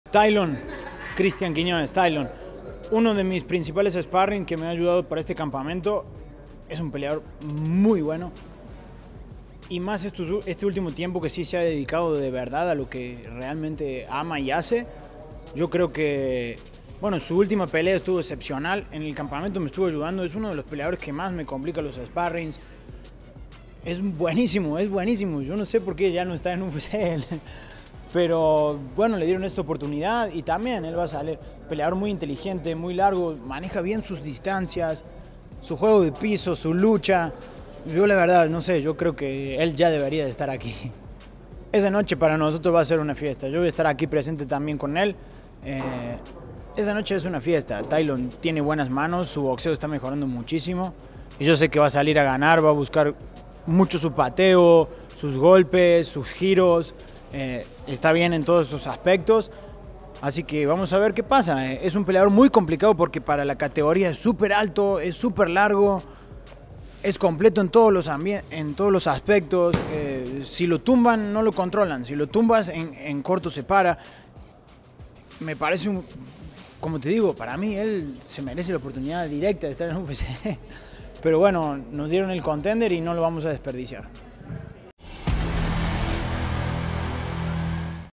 DWCS: Marce Rojo Apoya A Cristian Quiñonez
El peleador argentino Marcelo Rojo habló de su compañero de equipo, el mexicano Cristian Quiñonez en DWCS.